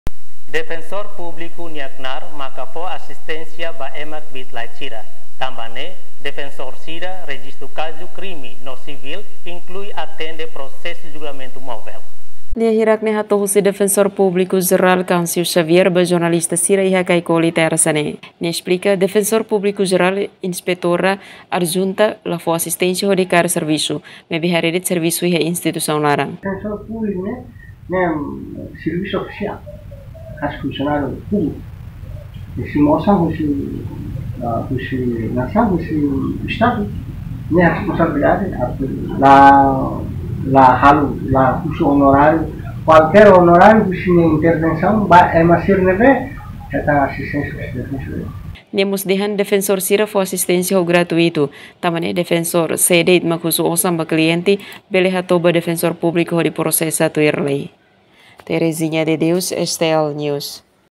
Defensor Atende Kazu Sivil No Krime
Defensor Publiku Nia Knar Maka Fo Asistensia Ba Ema Kbiit Laek Sira, Tanba Nee Defensor Sira Rejista Kazu Krime No Sivil, Inklui Atende Prosesu Julgamentu Movel.